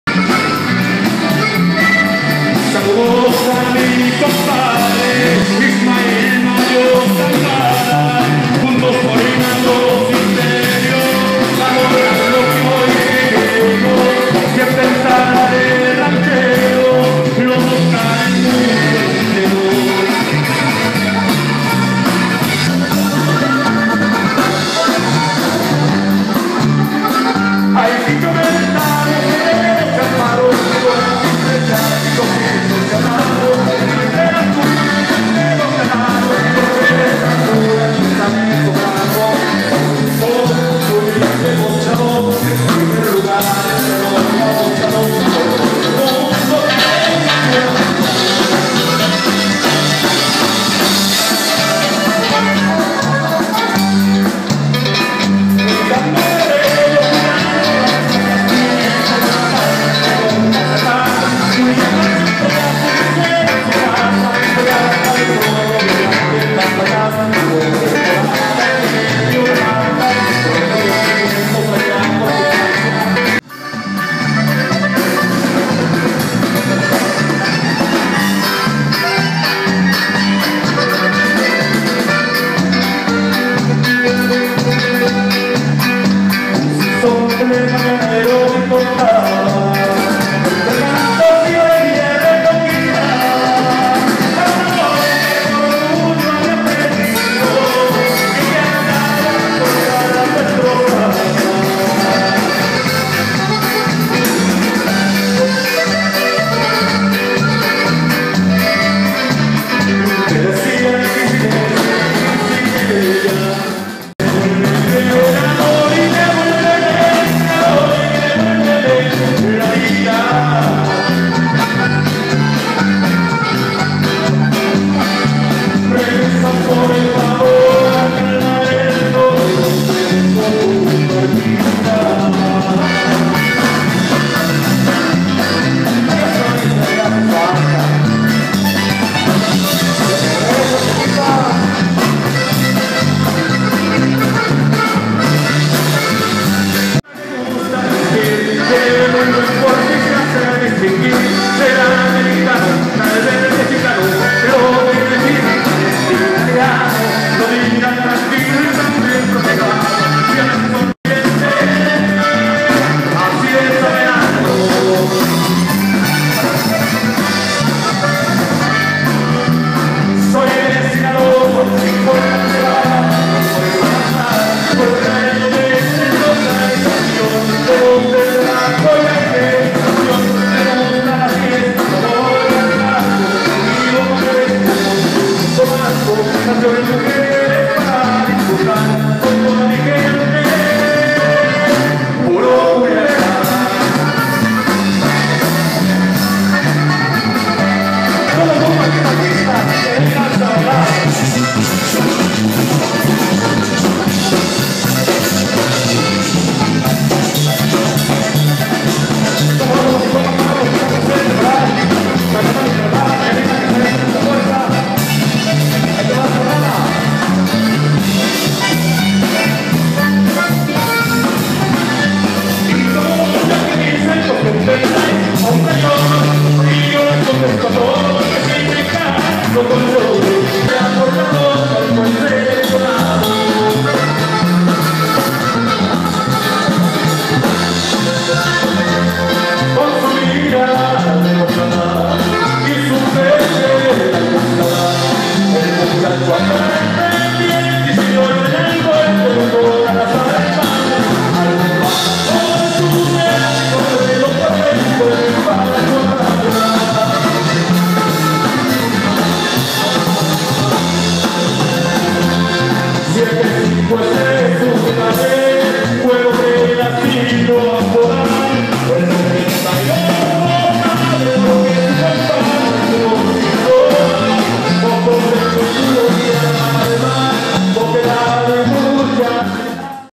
CHALO MARTINEZ En Vivo!! con El Grupo CUATRO LEYES
AKI ESTA SU COMPA CHALO MARTINEZ EN VIVO TOKANDO PUROS CORRIDOS DE SU AUTORIA ACOMPANADO DEL GRUPO CUATRO LEYES